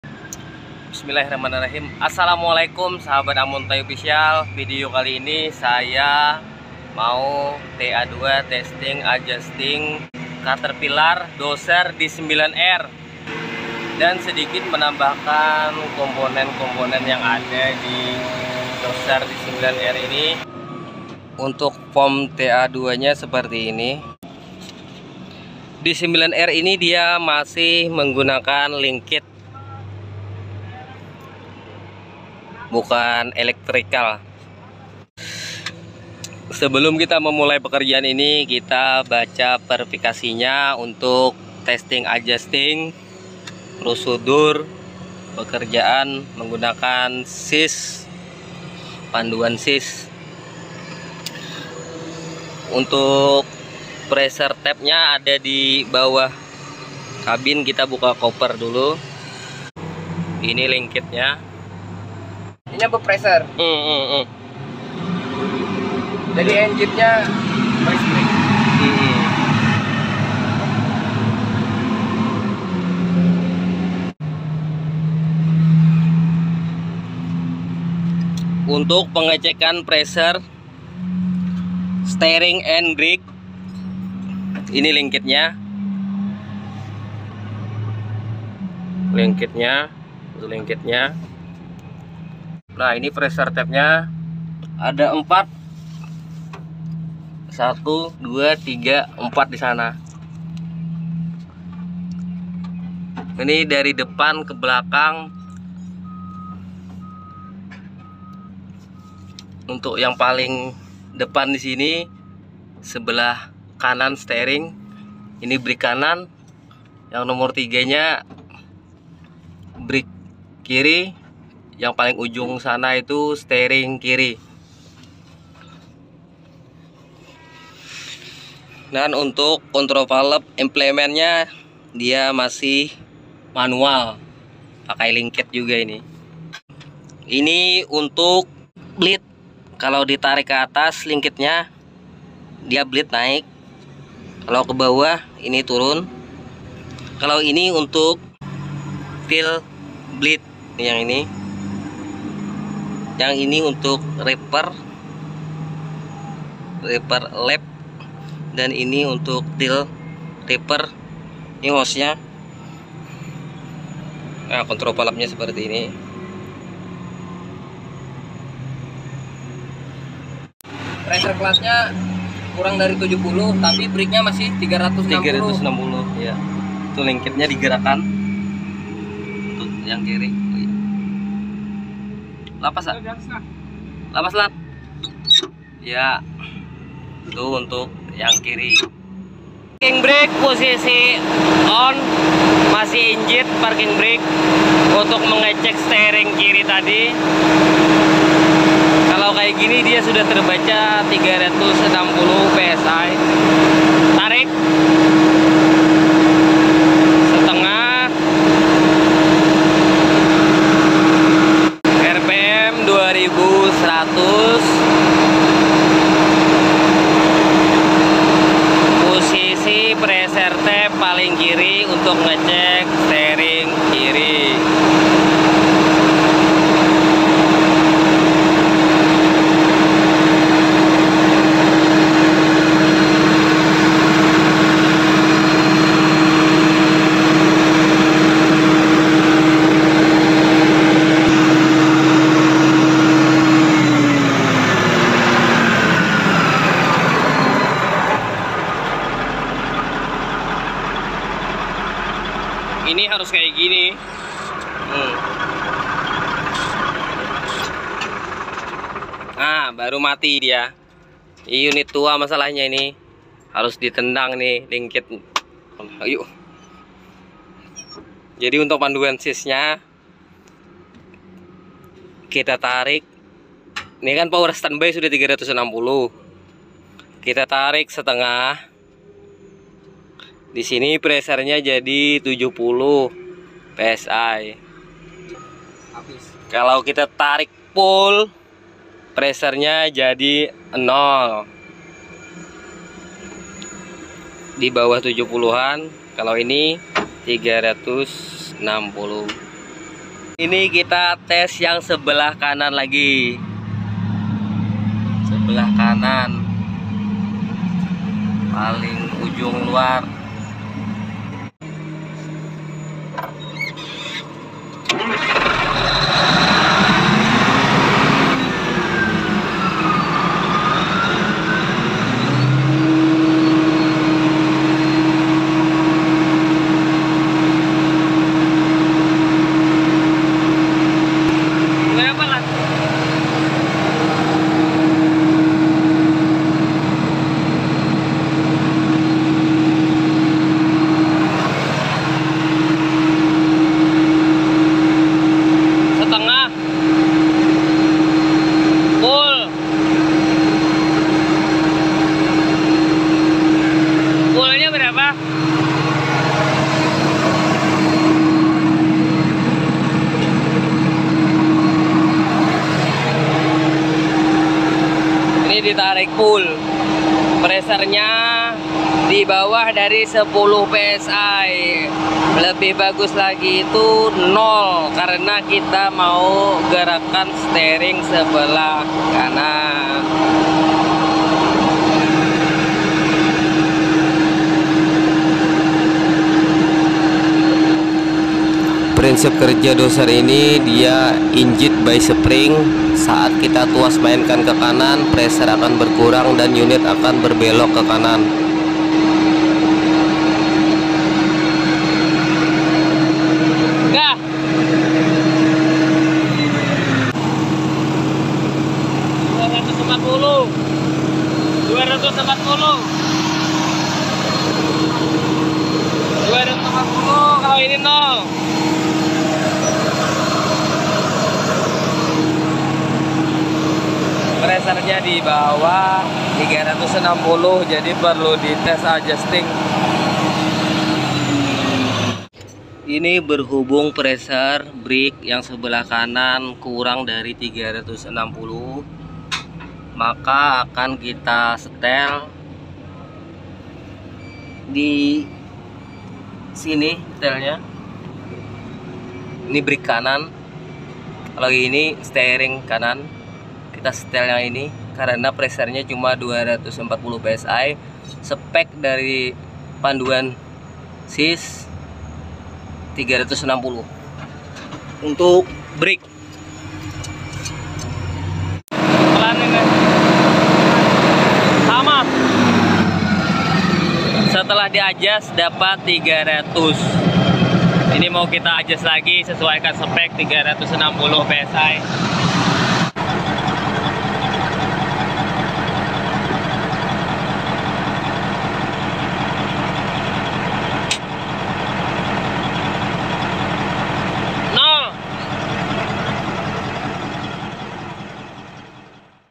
Cara setel rem dan kemudi pada alat berat dozer caterpillar d9r
Cara setel rem dan kemudi pada alat berat dozer caterpillar d9r menggunakan alat pengukur tekanan.

Video asli Amuntai official A20
Jangan unggah ulang video saya tanpa memberitahu saya.